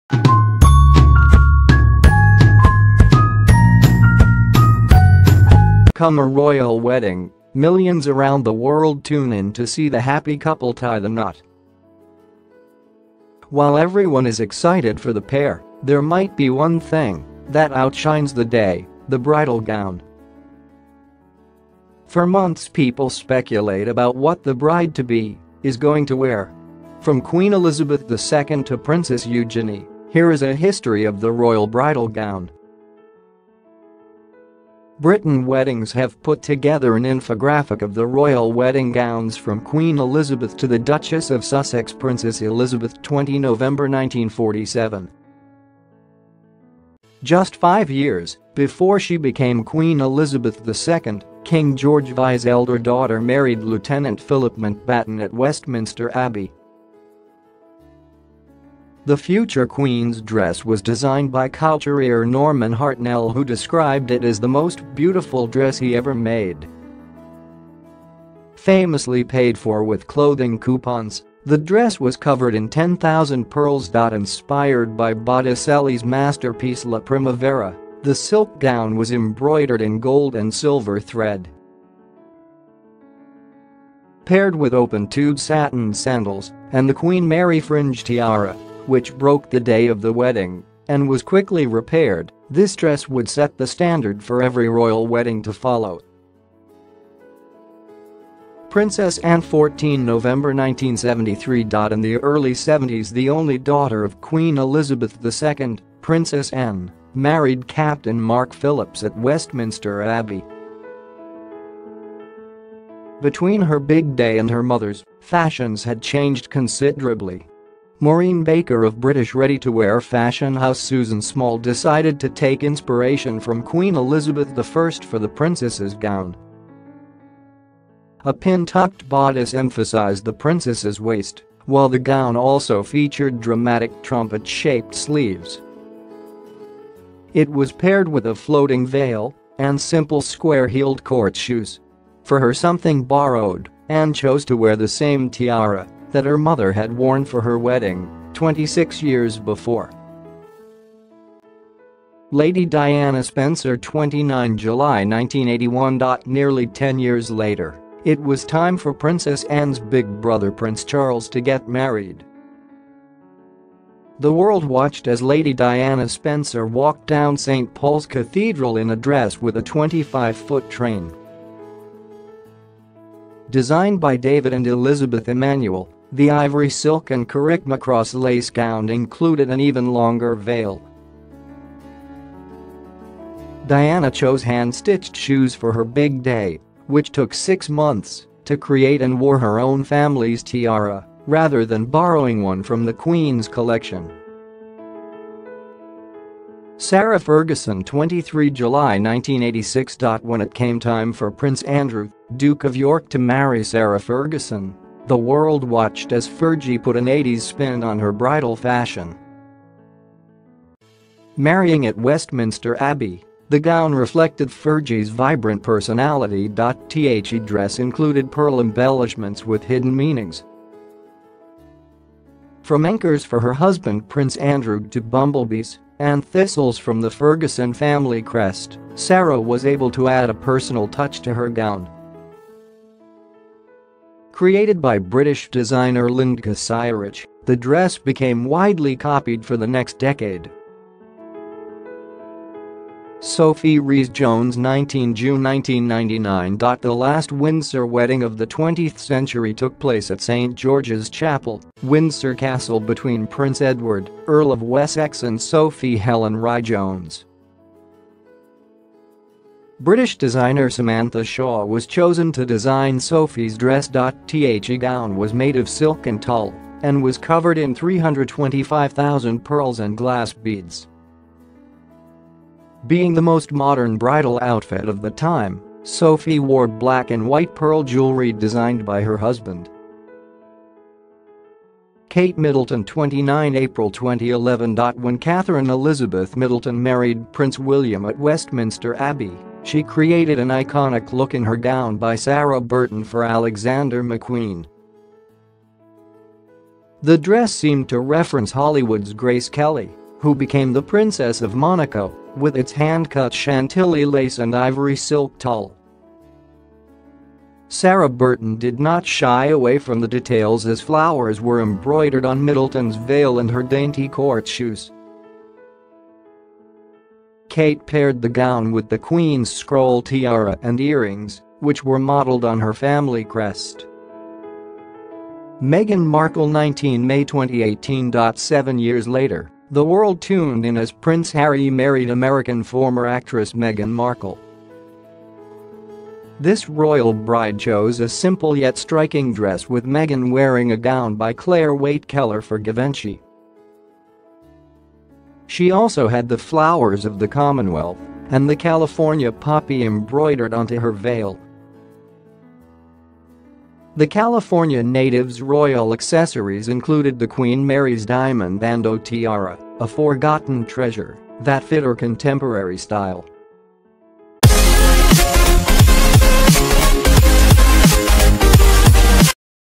British royal wedding gowns through the years-Royal news
Come a royal wedding, millions around the world tune in to see the happy couple tie the knot.  While everyone is excited for the pair, there might be one thing that outshines the day –  the bridal gown.  For months people speculate about what the bride-to-be is going to wear. From Queen Elizabeth II to Princess Eugenie, here is a history of the royal bridal gown. Britten weddings have put together an infographic of the royal wedding gowns from Queen Elizabeth to the Duchess of Sussex:Princess Elizabeth – 20 November 1947 . Just five years before she became Queen Elizabeth II, King George VI’s elder daughter married Lieutenant Philip Mountbatten at Westminster Abbey. The future Queen’s dress was designed by couturier Norman Hartnell who described it as the most beautiful dress he ever made.  Famously paid for with clothing coupons, the dress was covered in 10,000 pearls.Inspired by Botticelli’s masterpiece ‘La Primavera,’ the silk gown was embroidered in gold and silver thread.  Paired with open-toed satin sandals and the Queen Mary Fringe Tiara (which broke the day of the wedding and was quickly repaired), this dress would set the standard for every royal wedding to follow. Princess Anne – 14 November 1973 .In the early Seventies the only daughter of Queen Elizabeth II, Princess Anne, married Captain Mark Phillips at Westminster Abbey.  Between her big day and her mother’s, fashions had changed considerably. Maureen Baker of British ready to wear fashion house Susan Small decided to take inspiration from Queen Elizabeth I for the Princess’s gown. A pin tucked bodice emphasised the Princess’s waist while the gown also featured dramatic trumpet-shaped sleeves.  It was paired with a floating veil and simple square heeled court shoes. For her “something borrowed,” Anne chose to wear the same tiara that her mother had worn for her wedding, 26 years before. Lady Diana Spencer – 29 July 1981 .Nearly ten years later, it was time for Princess Anne’s big brother Prince Charles to get married.  The world watched as Lady Diana Spencer walked down St Paul’s Cathedral in a dress with a 25-foot train.  Designed by David and Elizabeth Emmanuel, the ivory silk and Carrickmacross lace gown included an even longer veil. Diana chose hand-stitched shoes for her big day, which took six months to create and wore her own family’s tiara rather than borrowing one from The Queen’s collection. Sarah Ferguson – 23 July 1986 .When it came time for Prince Andrew, Duke of York to marry Sarah Ferguson, the world watched as ‘Fergie’ put an 80s spin on her bridal fashion.  Marrying at Westminster Abbey, the gown reflected Fergie’s vibrant personality.The dress included pearl embellishments with hidden meanings.  From anchors for her husband Prince Andrew to bumblebees and thistles from the Ferguson family crest, Sarah was able to add a personal touch to her gown.  Created by British designer Lindka Cierach, the dress became widely copied for the next decade. Sophie Rhys-Jones – 19